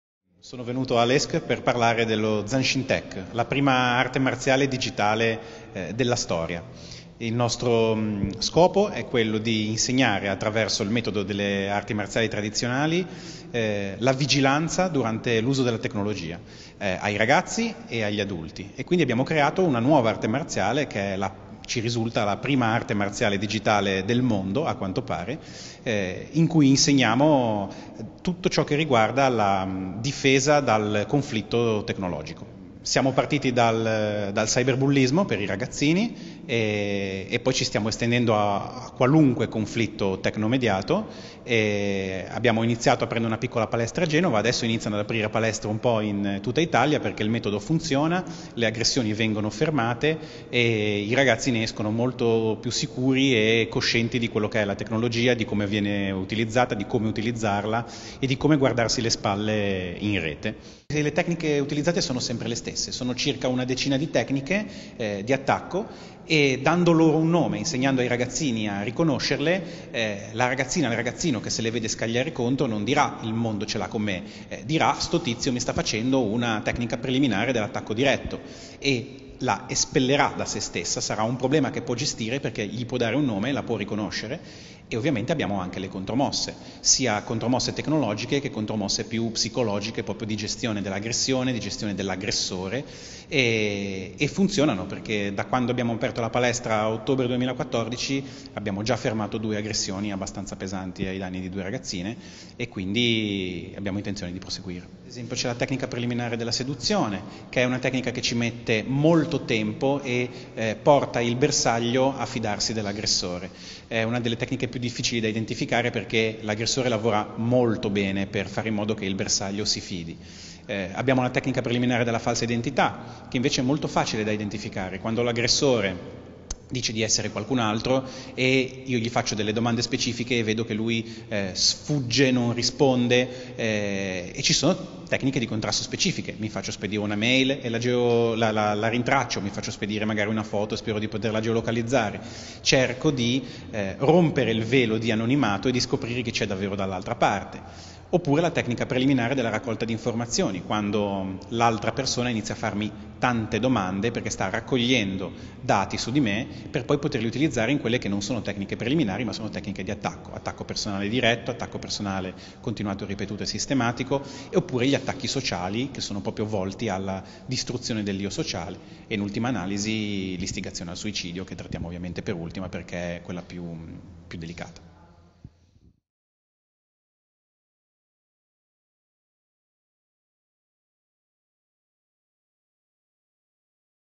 Zanshin Tech: la Prima Arte Marziale Digitale -Claudio 'CoD' Canavese
Con lo scopo di creare dei corsi di autodifesa digitale per ragazzi, i fondatori di Zanshin Tech hanno applicato l'impostazione e la disciplina delle arti marziali per codificare le tecniche di attacco, e quindi di difesa, nelle comunicazioni tecnomediate. Il risultato è un originale percorso formativo che mira a far acquisire ai ragazzi consapevolezza delle minacce e capacità di gestione del pericolo (Riprese ed editing video: Interlogica)